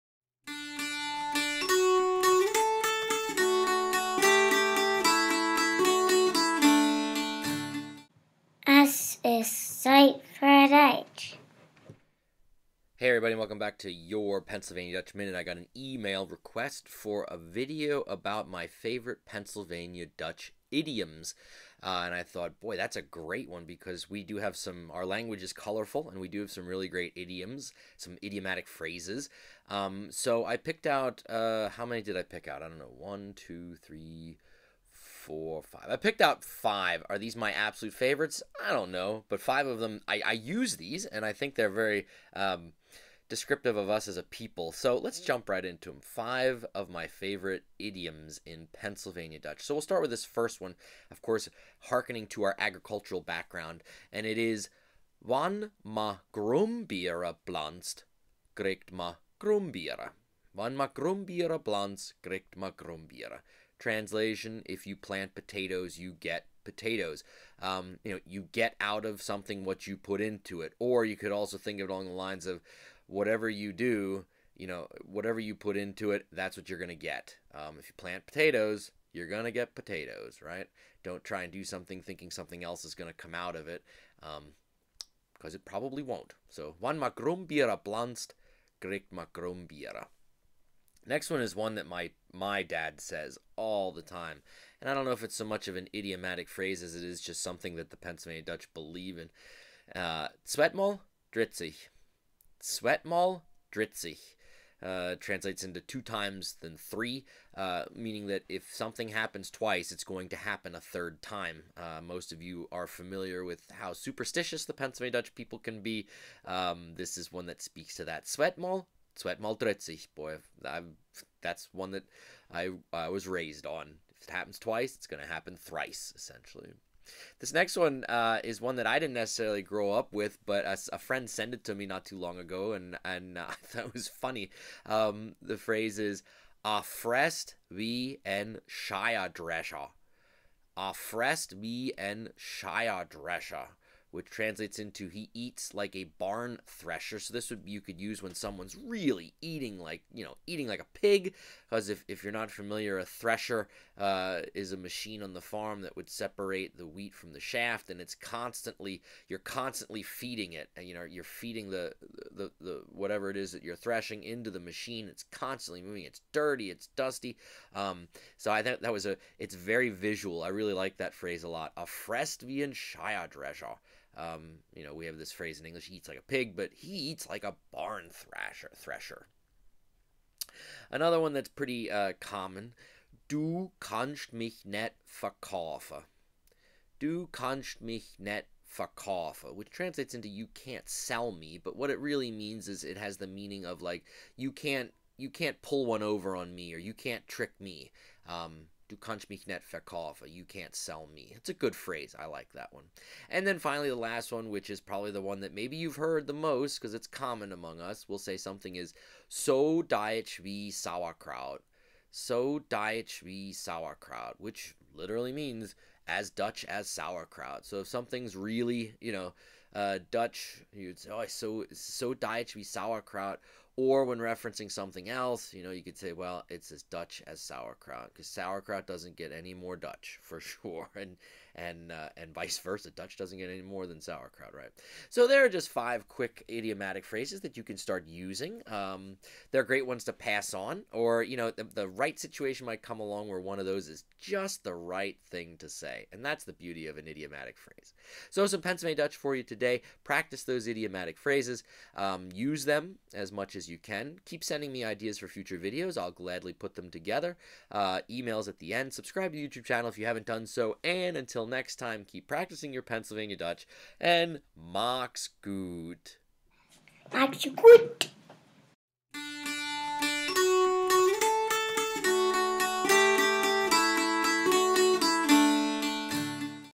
Your PA Dutch Minute: Idioms
This episode covers 5 different idiomatic phrases in PA Dutch.